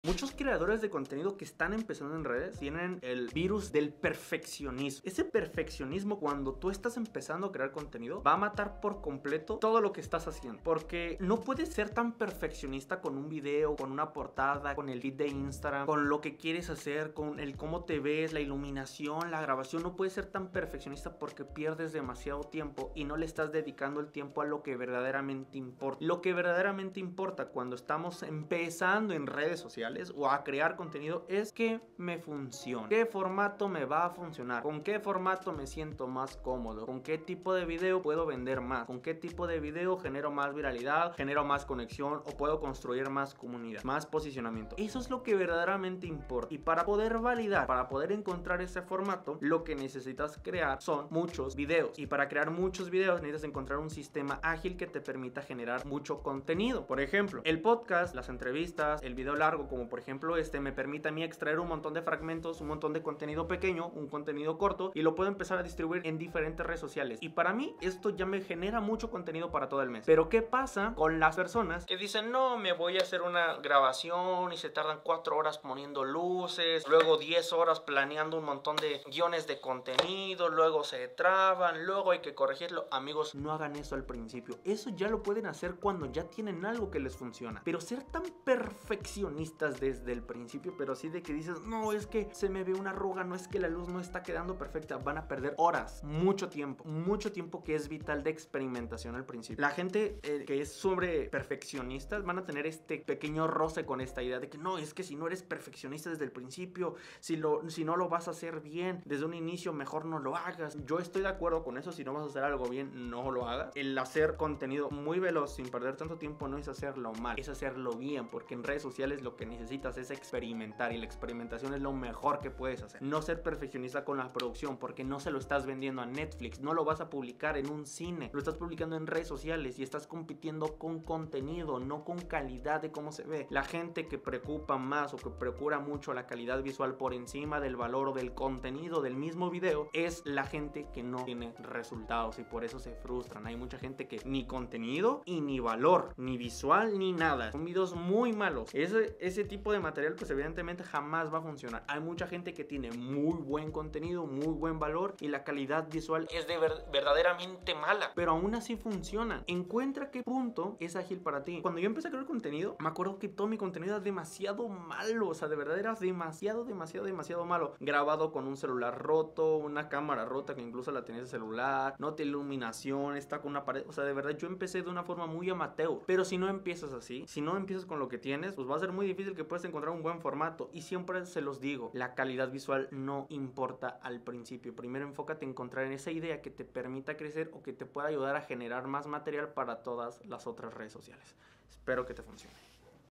¿Por qué NO TENGO VISTAS en mis videos? El problema que no te deja monetizar 😩
🐜Hormigas!
Este es uno de los problemas más comunes que tienen todos los creadores de contenido.